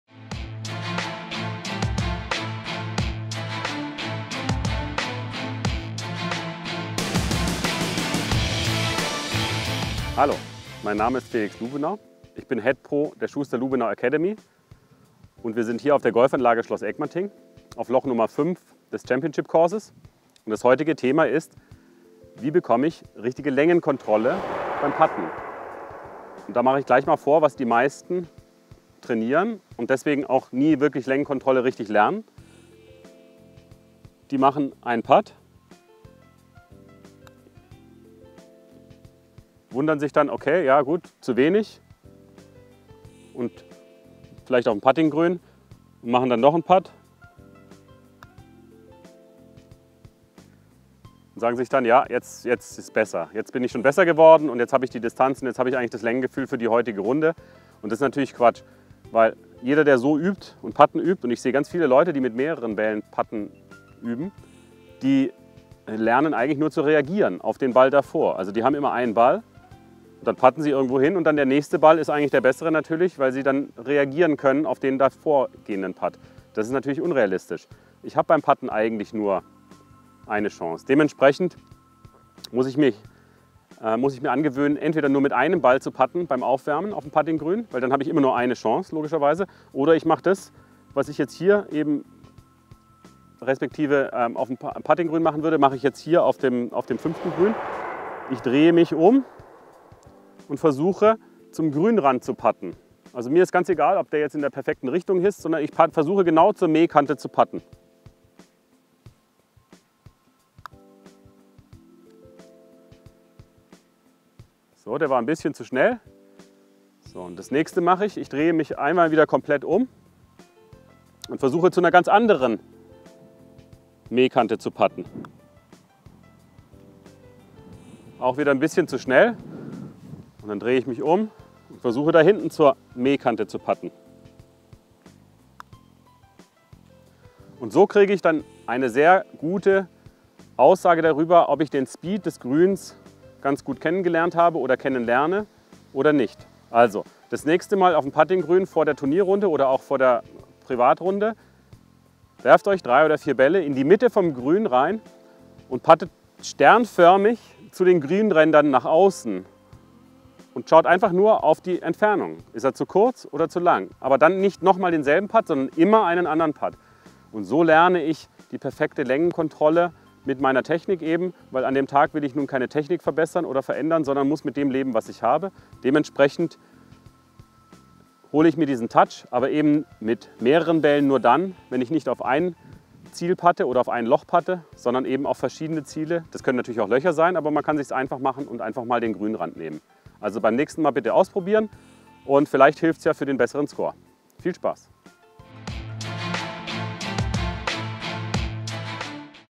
Golf Tipp: Längenkontrolle am Grün
Wie putte ich mich vor einer Runde richtig ein? Wie übe ich Längenkontrolle? Auf jeden Fall nicht in dem man sich zehn Bälle auf das Grün schmeisst und zu einer Fahne puttet. Felix Lubenau zeigt euch wie es effektiver geht.